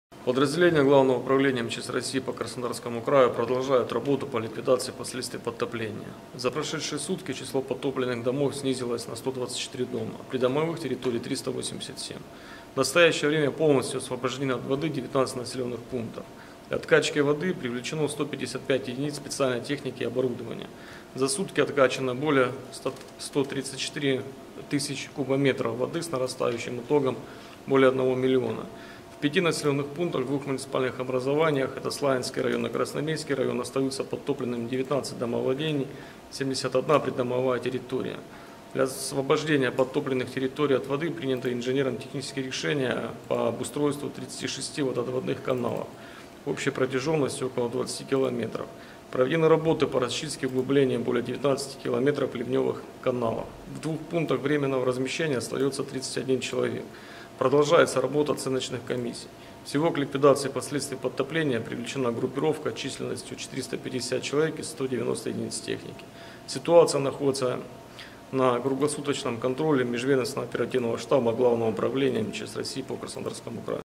МЧС продолжают работы по ликвидации последствий подтоплений
Комментарий Старшего оперативного дежурного ЦУКС Главного управления Алексея Сальника.

Подразделения Главного управления МЧС России по Краснодарскому краю продолжают работу по ликвидации последствий подтоплений. За прошедшие сутки число подтопленных домов снизилось на 124, придомовых территорий - на 387. В настоящее время полностью освобождено от воды 19 населенных пунктов.

Для откачки воды привлечено 155 единиц специальной техники и оборудования.

За сутки откачано более 134-х тысяч кубометров, с нарастающим итогом более 1 млн. 100 тысяч кубометров.
В 5-ти населенных пунктах 2-х муниципальных образований - в Славянском и Красноармейском районах, остаются подтопленными 19 домовладений и 71 придомовая территория.

Для освобождения подтопленных территорий от воды приняты инженерно-технические решения по обустройству 36 водоотводных каналов, общей протяженностью около 20 км, проведены работы по расчистке и углублению более 19 км ливневых каналов.
В 2-х пунктах временного размещения остается 31 человек.

Продолжается работа оценочных комиссий.
Всего к ликвидации последствий подтоплений привлечена группировка численностью 450 человек и 190 единиц техники.

Ситуация находится на круглосуточном контроле межведомственного оперативного штаба Главного управления МЧС России по Краснодарскому краю.